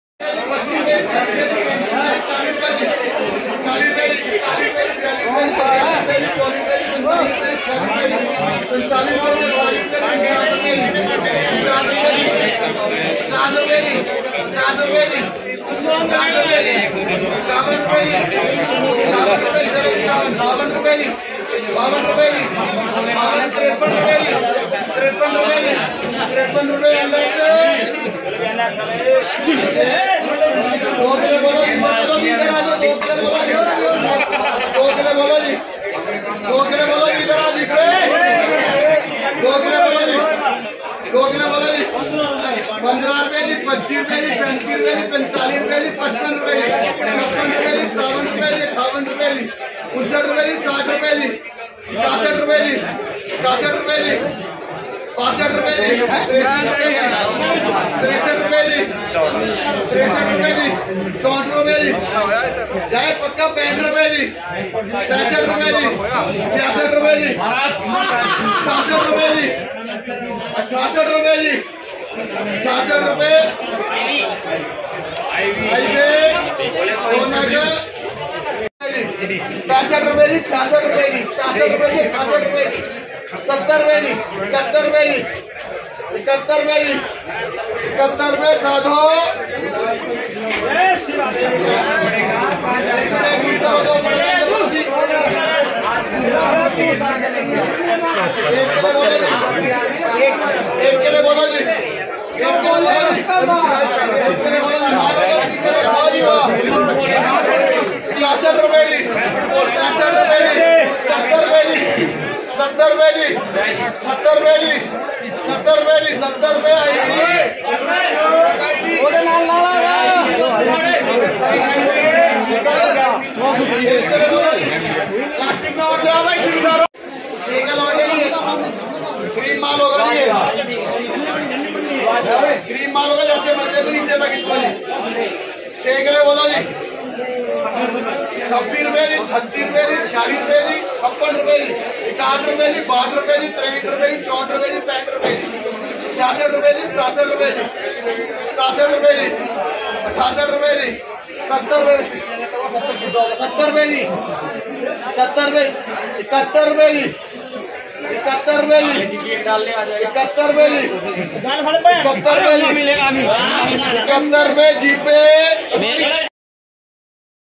शिमलामिर्च बोली लाइव।। 75 रुपये तक बिकी शिमला मिर्च।। सोलन मंडी हिमाचल प्रदेश।। 17 सितंबर 2024
शिमलामिर्च बोली लाइव।। 75 रुपये तक बिकी शिमला मिर्च।। सोलन मंडी हिमाचल प्रदेश।। 17 सितंबर 2024 किसान न्यूज Kisaan News